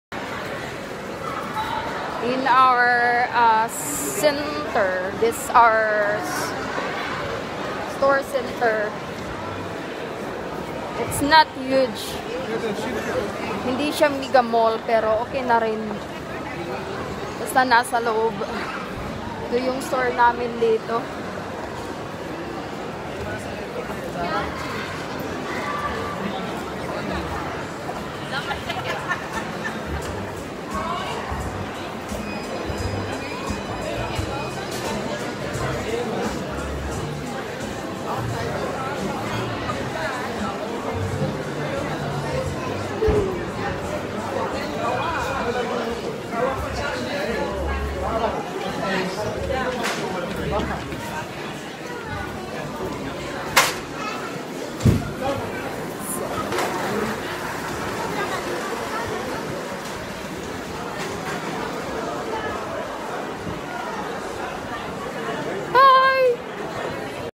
DIGITAL WINKEL LOUNGE AMSTELVEEN HET MOOISTE WINKEL MET GRATIS WIFI
I call this winkel or department store as a digital lounge store because in the heart inside of this store you feel like your nearest to the schiphol or feeling like Airport one stop lounger where you can freely use the wifi , Amstelveen in my choosen beautiful winkel or store with the chique outlook of the interior designed. If you want to experience the different way of shopping come to this city and enjoy the shopping just one punch of your digital shopping Amstelveen Card  Experience no hassle no bussle, Try also to experience the biblioteek coffee  sit back and relax .

(not a sponsor) Amstelveen Winkel visit here for hightech experience .location:The Netherlands